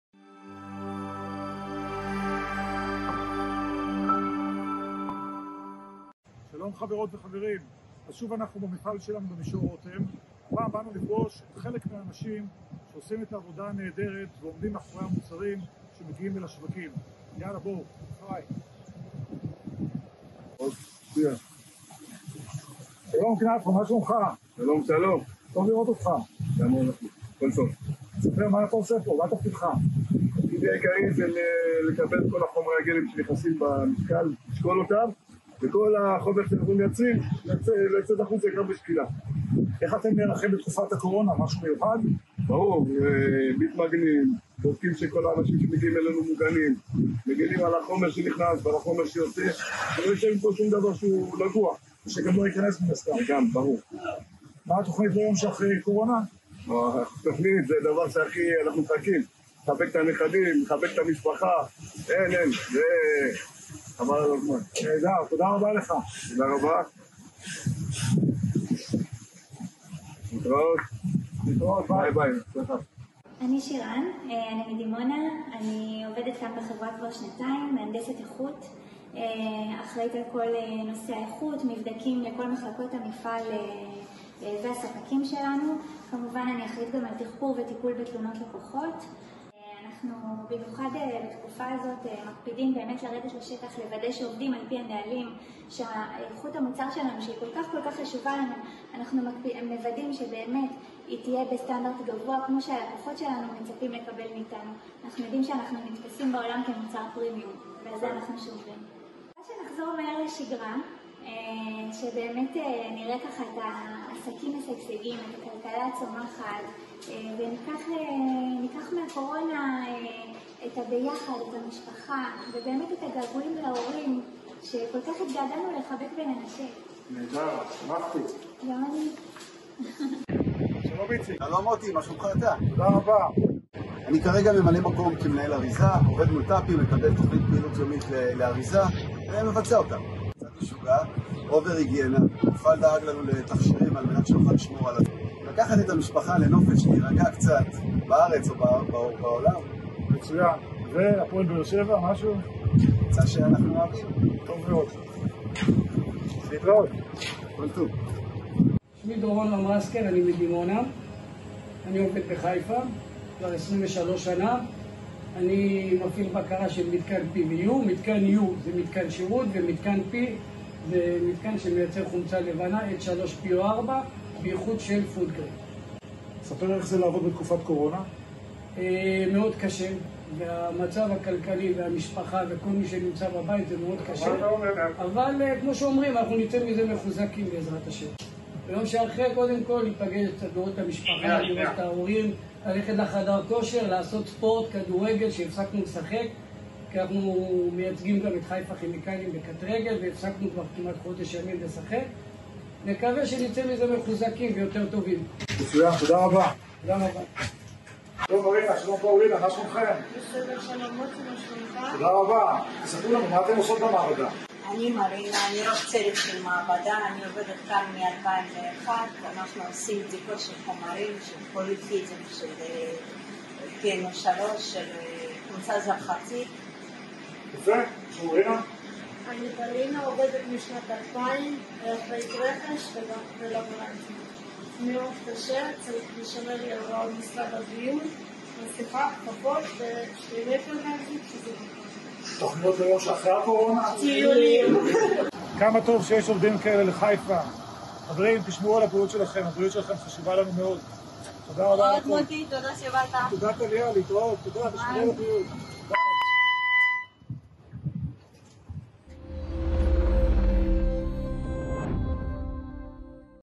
Factory Visit during epidemic
Motti Levin, Haifa Group CEO
Went last Thursday to visit the factory workers in Mishor Rotem, and heard how they are coping with the epidemic situation of Coronavirus, while maintaining responsible for producing and delivering most of the goods Haifa sells worldwide.